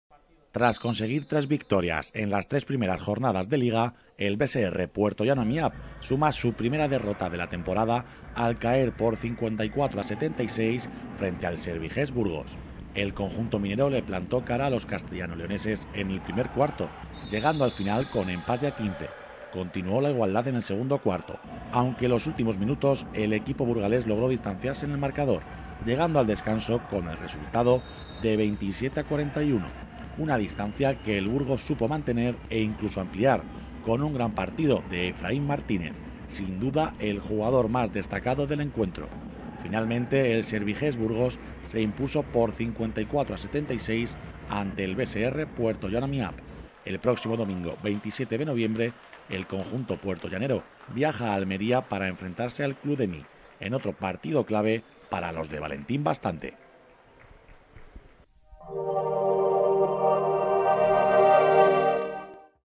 Crónica BSR Puertollano Amiab 54 - 76 Servigest Burgos
Tras conseguir tres victorias en las tres primeras jornadas de liga, el BSR Puertollano Amiab suma su primera derrota de la temporada al caer por 54 a 76 frente al Servigest Burgos. El conjunto minero le plantó cara a los castellano-leoneses en el primer cuarto, llegando al final con empate a 15. Continuó la igualdad en el segundo cuarto, aunque en los últimos minutos el equipo burgalés logró distanciarse en el marcador, llegando al descanso con el resultado de 27 a 41. Una distancia que el Burgos supo mantener e incluso ampliar, con un gran partido de Efrain Martínez, sin duda el jugador más destacado del encuentro. Finalmente, el Servigest Burgos se impuso por 54 a 76 ante el BSR Puertollano Amiab. El próximo domingo 27 de noviembre el conjunto puertollanero viaja a Almería para enfrentarse al Cludemi en otro partido clave para los de Valentín Bastante.